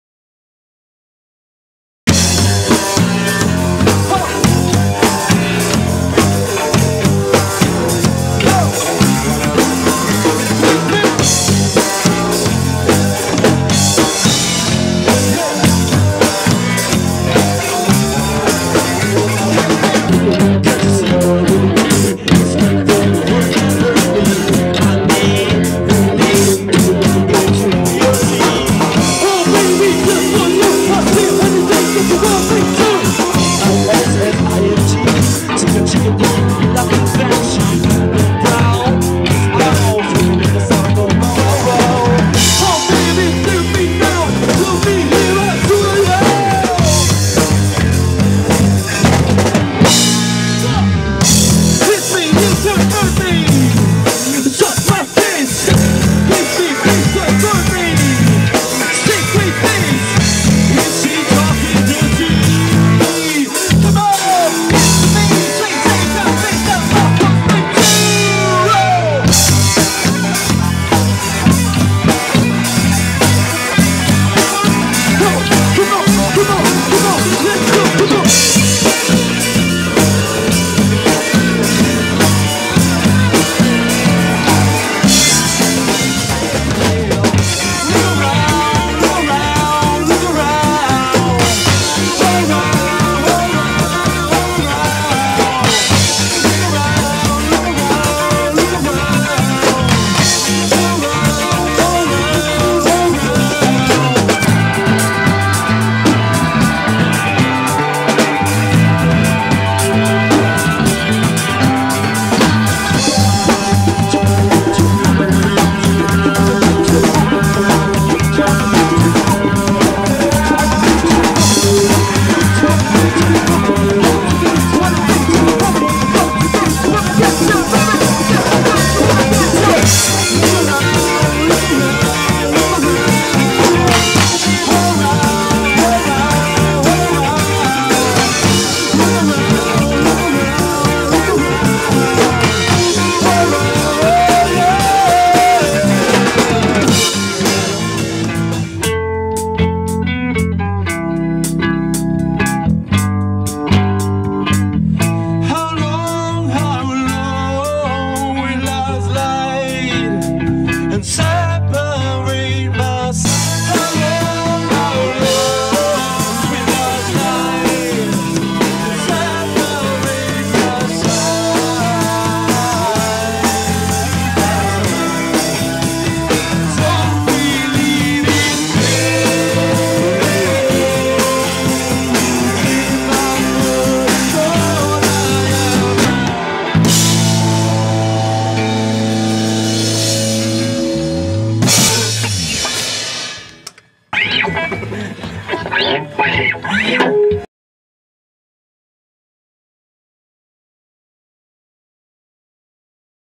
Trailer Mucca'S Milk Tribute Band Red hot chili peppers
La tribute band Pugliese dei mitici Red Hot Chili Peppers. "Mucca'S Milk"